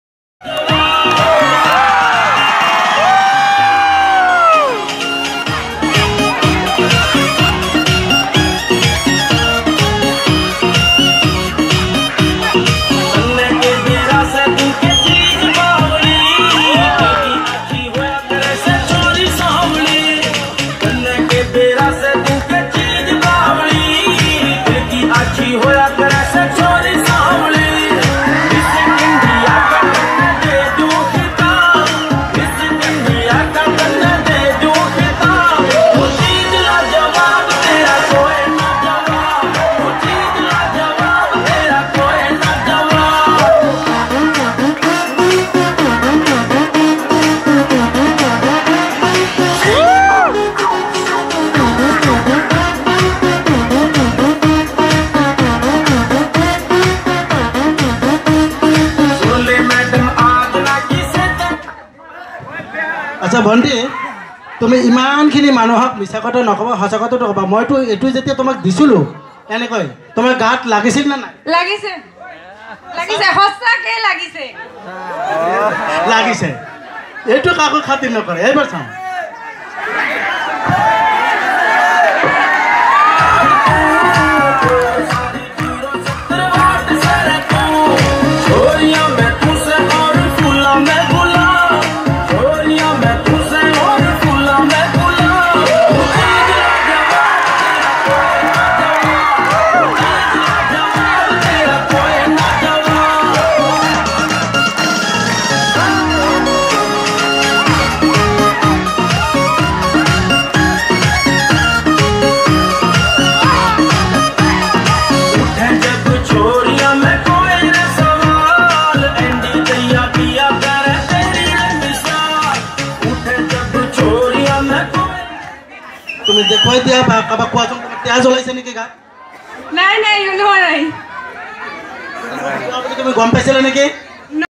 Magic Show
Really Very Interesting.It Is In Our School In H.N.Seminary H.S.School.